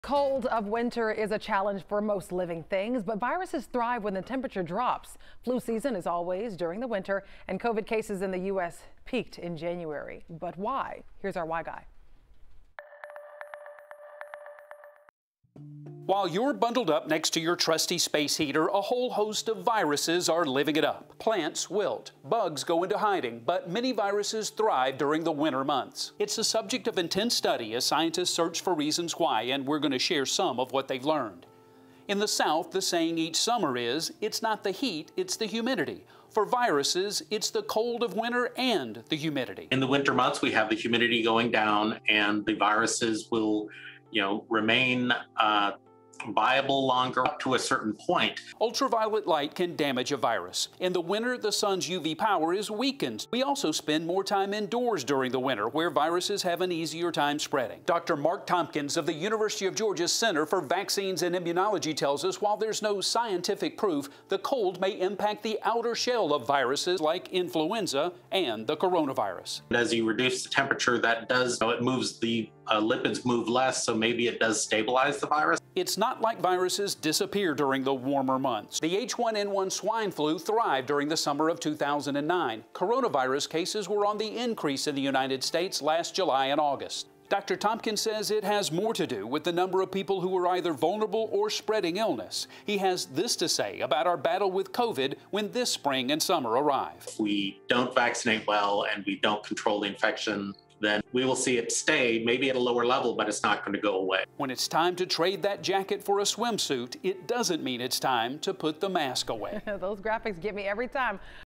Why do viruses thrive in the winter?
11Alive's Why Guy Jerry Carnes explains.